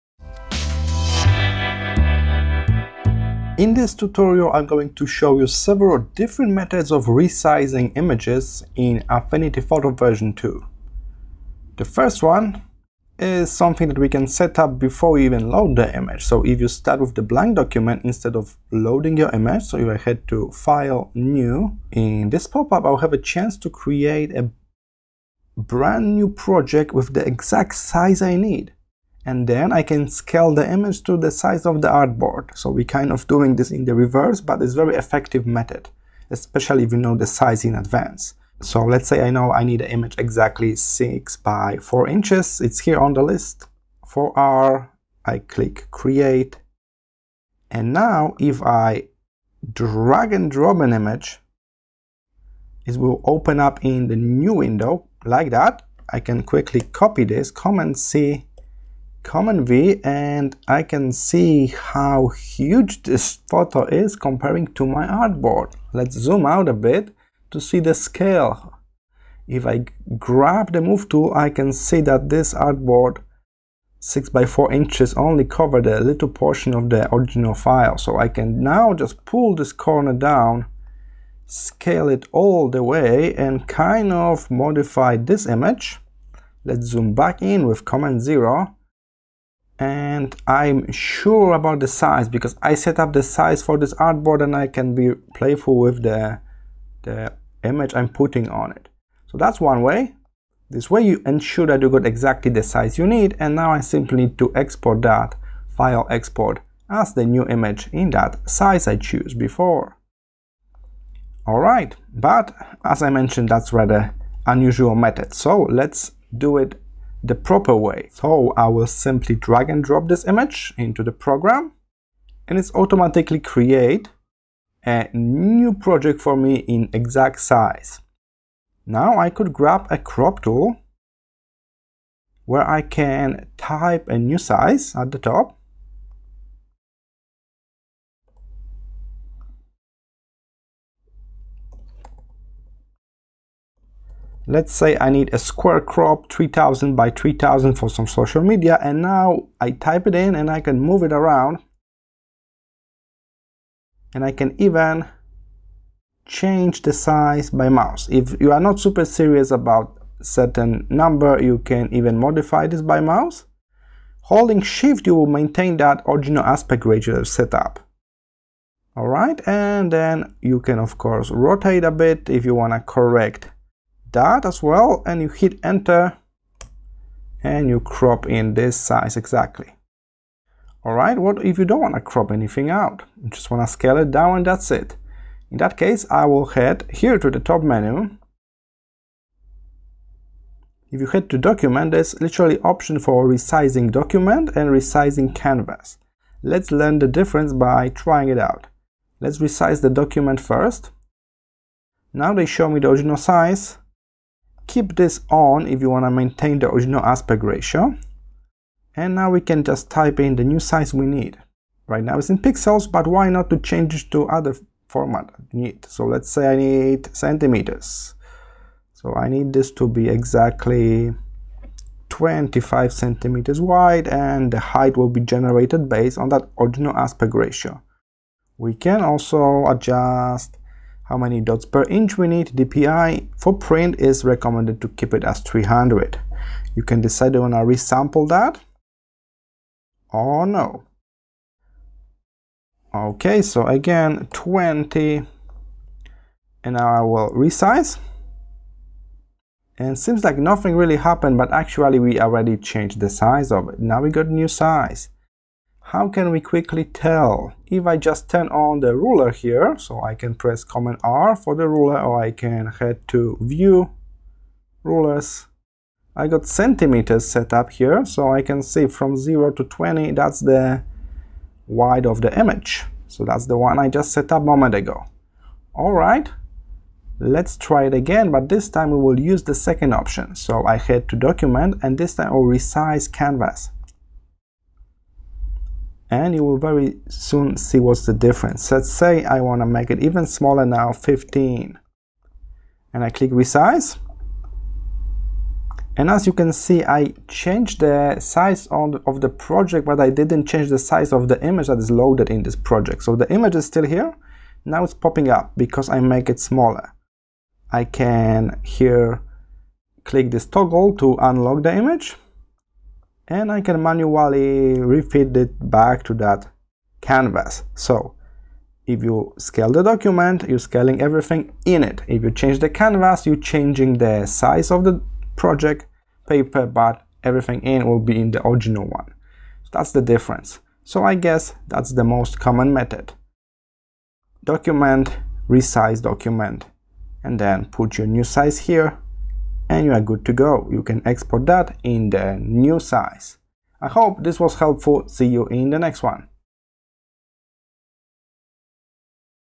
Affinity Photo How To Resize Images Tutorial
In this tutorial, I will show you how to resize images in Affinity Photo. Whether you're scaling down your image for web use or adjusting it for print, Affinity Photo makes the resizing process simple and precise. You will learn how to maintain the quality of your image while changing its dimensions, along with tips on aspect ratio and resolution settings to get the best results for your project.